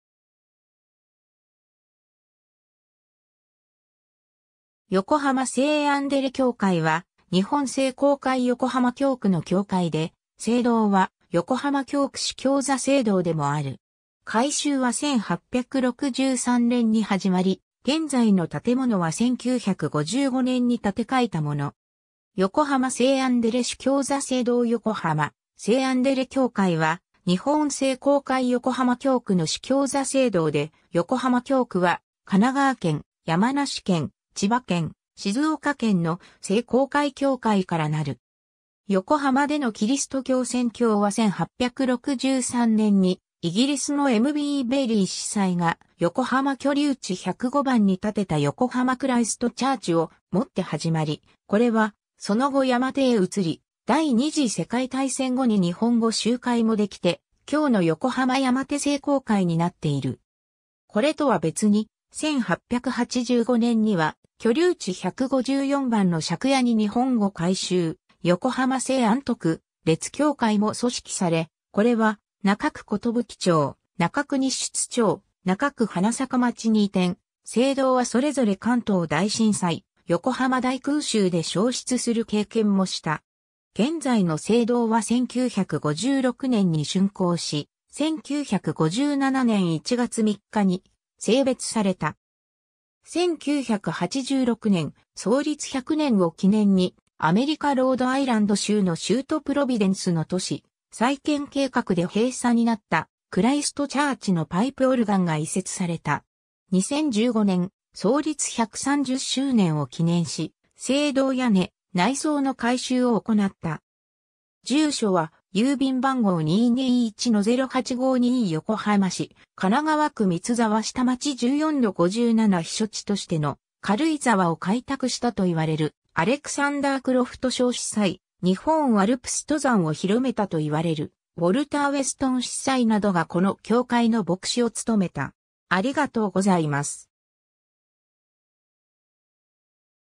聖アンデレ教会 (横浜)
横浜聖アンデレ教会（よこはませいアンデレきょうかい、英語：St. Andrew's Anglican Church, Yokohama）は、日本聖公会横浜教区の教会で、聖堂は横浜教区主教座聖堂でもある。
会衆は1863年に始まり、現在の建物は1955年（昭和30年）に建て替えたもの。
 横浜聖アンデレ主教座聖堂（正面） 横浜聖アンデレ教会は日本聖公会横浜教区の主教座聖堂で、横浜教区は神奈川県、山梨県、千葉県、静岡県の聖公会教会からなる。
 横浜でのキリスト教宣教は1863年にイギリスのM.B.ベイリー司祭が横浜居留地105番に建てた横浜クライスト・チャーチ（Yokohama Christ Church）をもって始まり、これはその後山手へ移り、第二次世界大戦後に日本語集会もできて、今日の横浜山手聖公会になっている。
 これとは別に、1885年（明治18年）には居留地154番（現在の中区山下町）の借家に日本語会衆「横浜聖安得烈教会」（横浜聖アンデレ教会）も組織され、これは、中区寿町・中区日出町・中区花咲町に移転、聖堂はそれぞれ関東大震災（1923年）・横浜大空襲（1945年）で焼失する経験もした。
現在の聖堂は1956年（昭和31年）に竣工し、1957年1月3日に聖別された。
 1986年、創立100年を記念に、アメリカ・ロードアイランド州の州都プロビデンスの都市再建計画で閉鎖になったクライストチャーチのパイプオルガン（1889年製）が移設された。
 2015年、創立130周年を記念し聖堂屋根、内装の改修を行った。
 住所は、〒221-0852　横浜市神奈川区三ツ沢下町14-57 避暑地としての軽井沢を開拓したといわれるアレクサンダー・クロフト・ショー司祭、日本アルプス登山を広めたといわれるウォルター・ウェストン司祭などがこの教会の牧師を務めた。